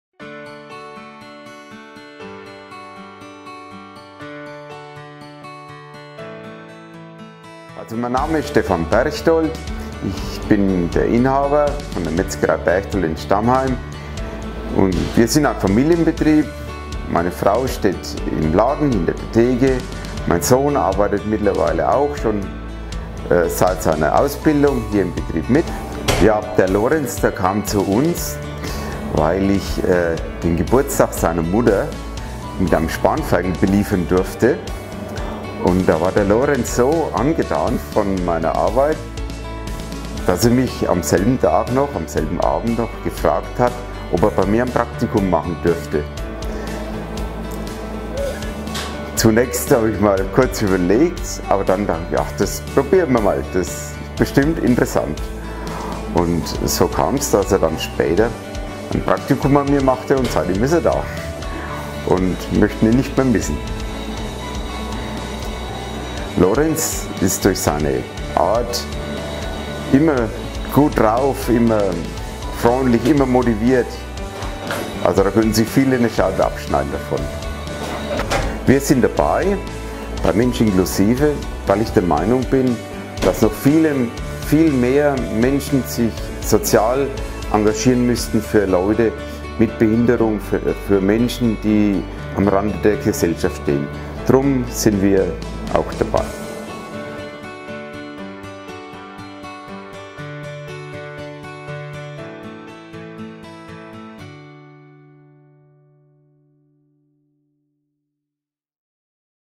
Image Film: Mensch inklusive - Metzgerei Berchtold
Die Metzgerei Berchtold in Stammheim ist dabei, beim exklusiven Netzwerk von "Mensch inklusive".